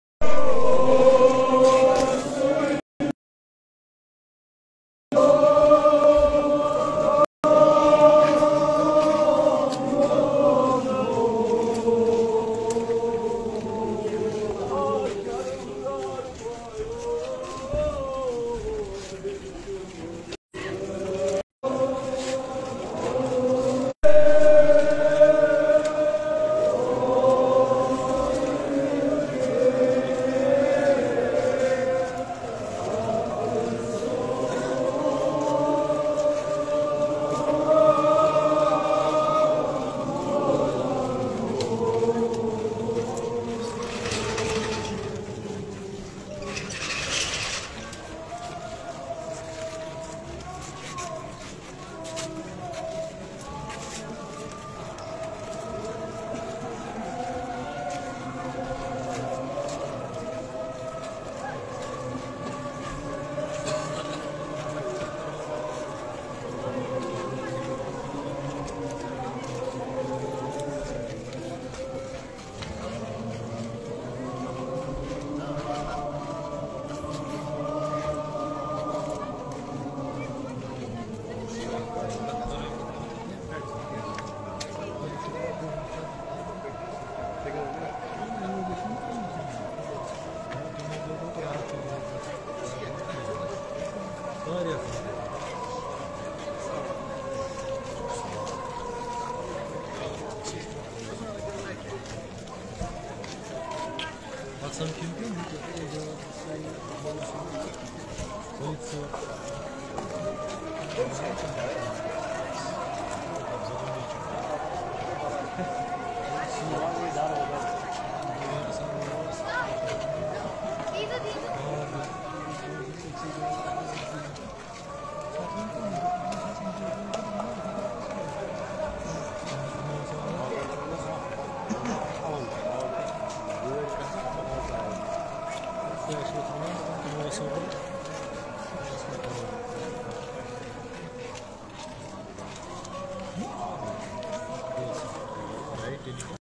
New Noha 2022 | 10th Muharram Jaloos e Aza At Zadibal Srinagar Kashmir | Muharram in Kashmir 2022
Camera's Used:
Canon 200D Mark II
Mic Used:
BOYA BY M1 Omnidirectional Lavalier Microphone
Tripod Used:
Benro T-880EX